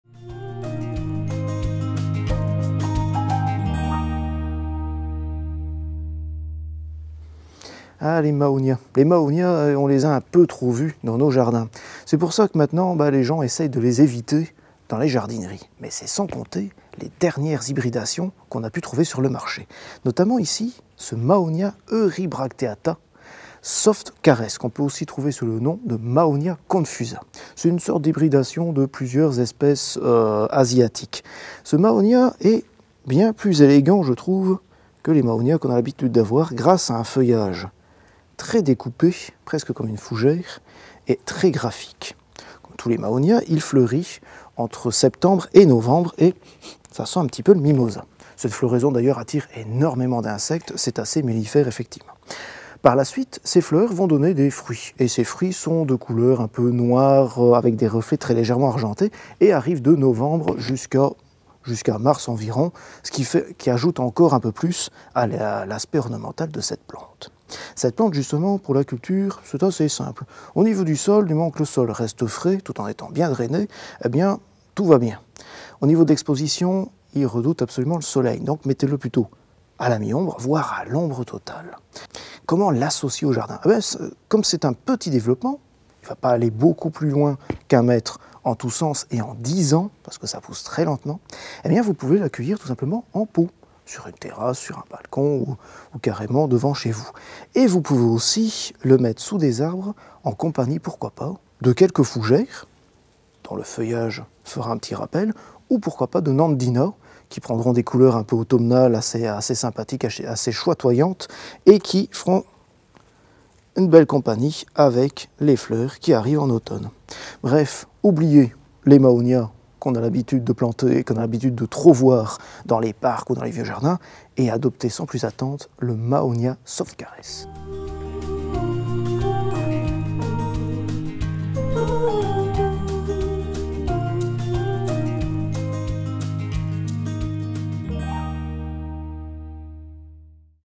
Découverte : le mahonia soft caress
Bonjour et bienvenue à tous!

Olivier nous présente aujourd'hui une variété de mahonia au feuillage doux : le mahonia "soft caress". Plantez le à l'ombre d'un arbre avec un nandina ou des fougères ou tout simplement en pot pour embellir votre jardin ou votre terrasse ! Une floraison qui rappelle le mimosa et qui se déroule de Septembre à Novembre le mahonia soft caress possède de nombreux atouts !

Si vous êtes séduit(e)s par cette plante réservez-la dès maintenant